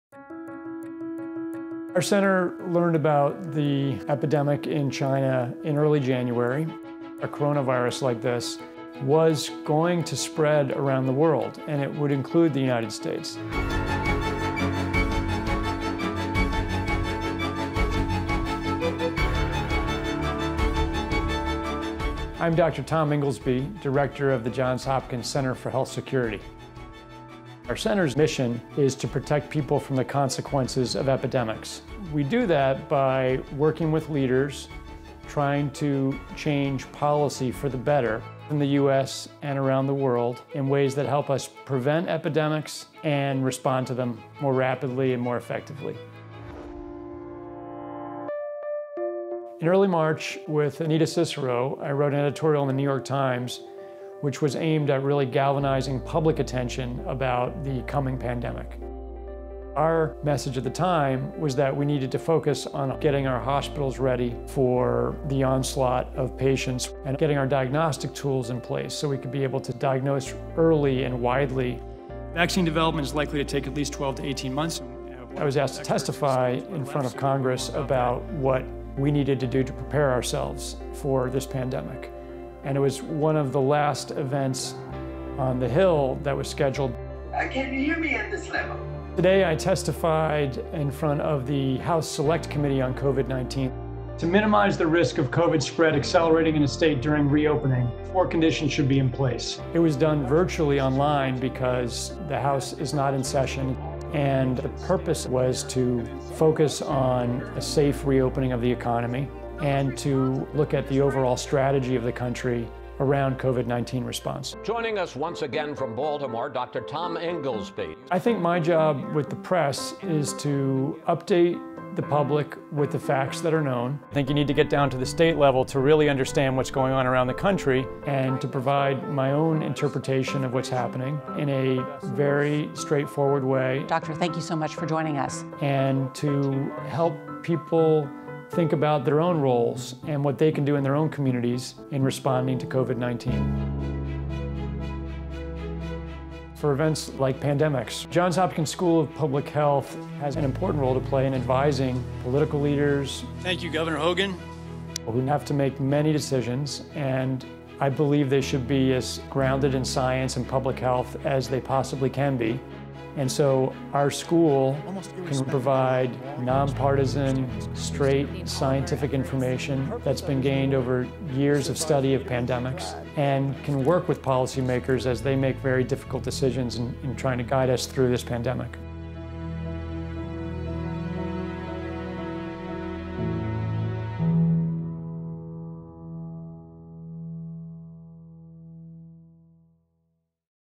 COVID-19 REAL-TIME RESPONSE: Dr. Tom Inglesby
"COVID-19 REAL-TIME RESPONSE: Stories from the Johns Hopkins Bloomberg School of Public Health Community" presents a video profile of Dr. Tom Inglesby, the director of the Johns Hopkins Center for Health Security.

From galvanizing the public to advising governments across the globe, Dr. Tom Inglesby has been at the forefront of the world's pandemic response.

This is a quick glimpse of his COVID19RealTimeResponse.

[Photos by Will Kirk / @JohnsHopkins]